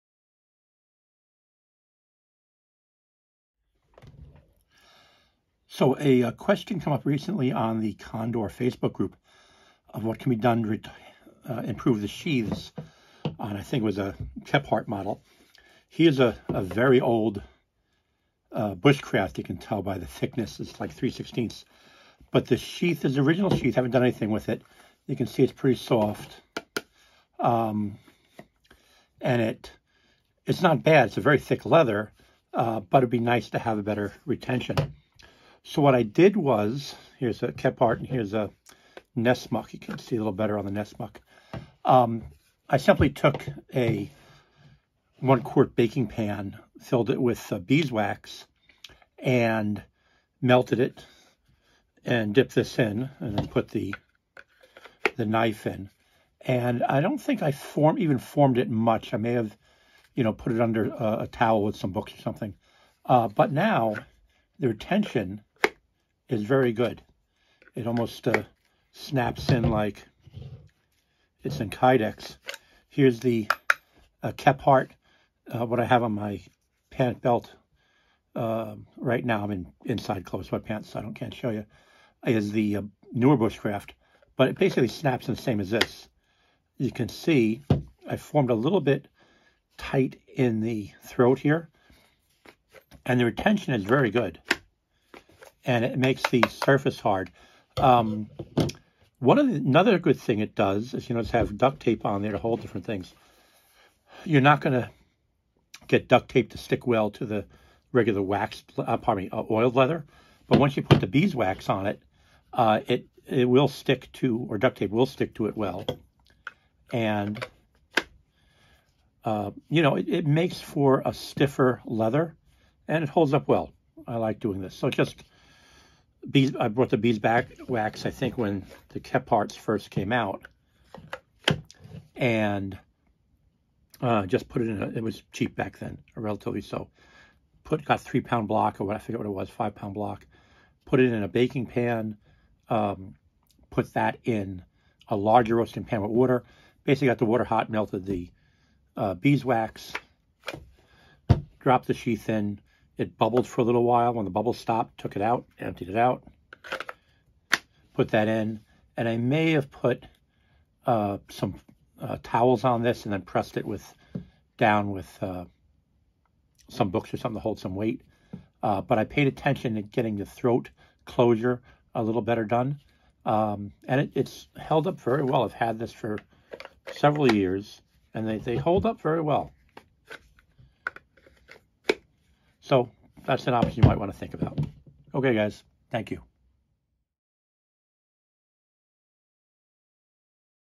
HOW TO WAX A LEATHER KNIFE SHEATH
How and why to wax a leather knife sheath.
The knives shown are all from Condor Knife & Tool:
Old model Bushcraft
Kephart
Nusmuk